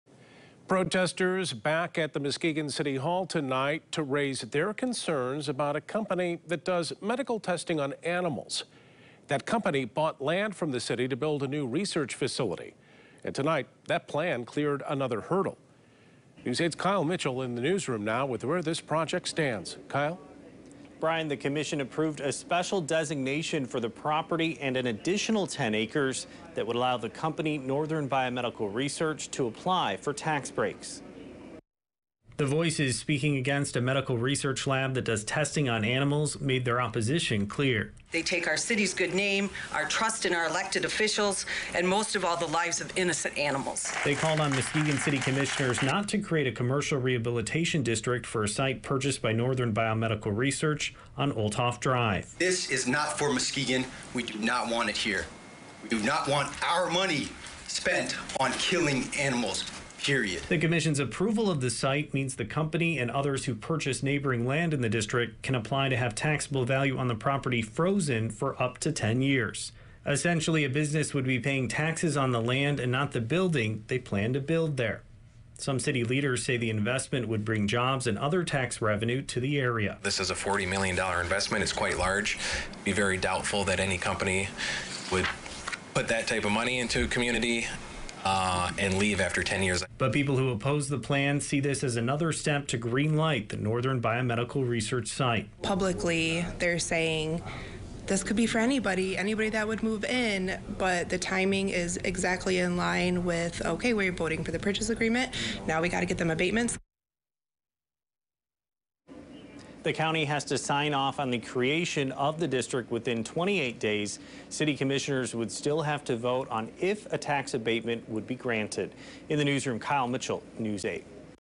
Proposed animal testing site draws protests at Muskegon commission meeting
Protesters returned to Muskegon City Hall Tuesday night to raise concerns about a company that does medical testing on animals. (March 22, 2022)